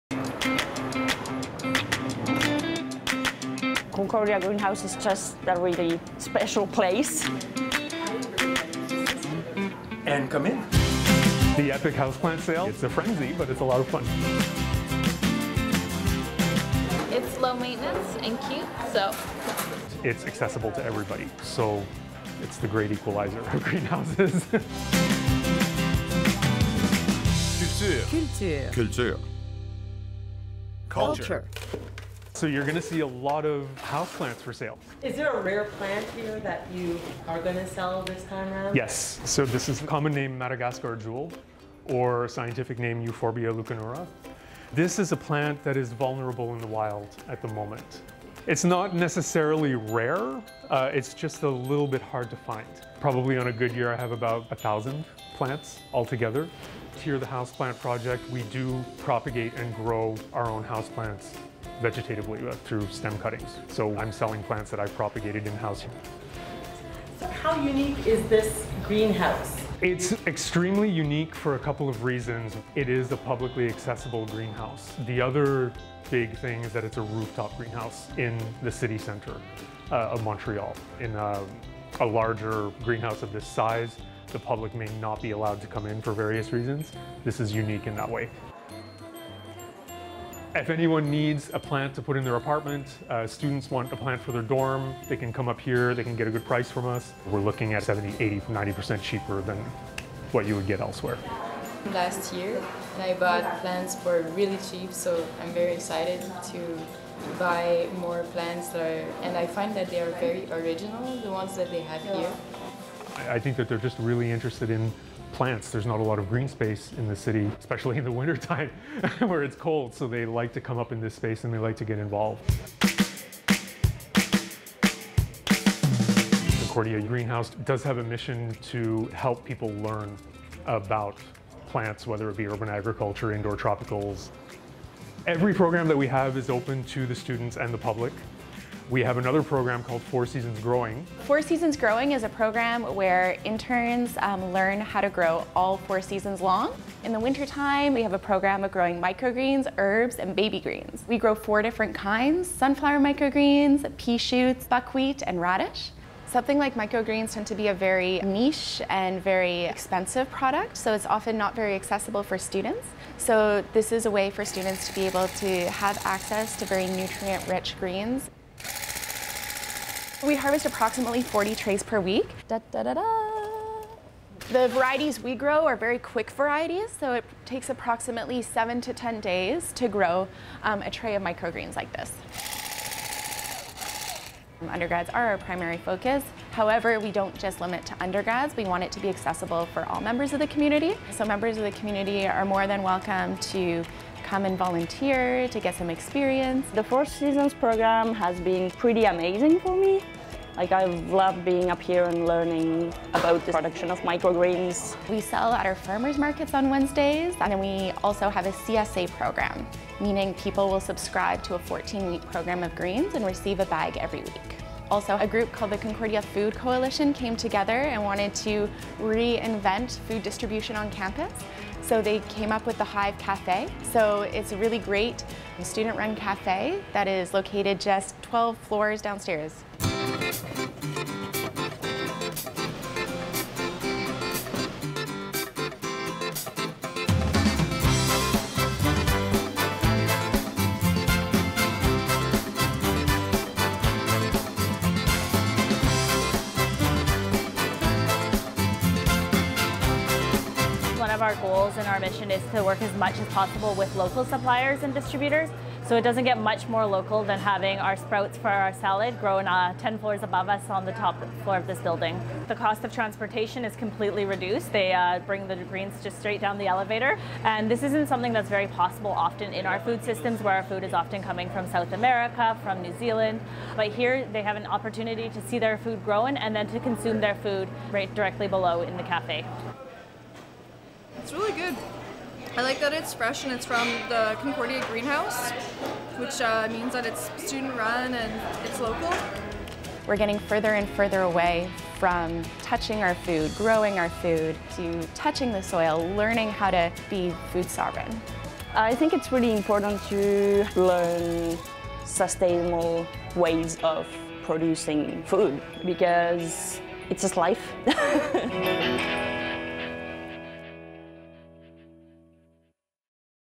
Qulture #303 Concordia Greenhouse
Visit the only publicly-accessible rooftop garden in downtown Montréal and learns about the programs it offers.

Qulture - (a mash-up of ‘Québec’ and Culture’) explores the thriving arts scene and unique artistic talent in and around Montréal.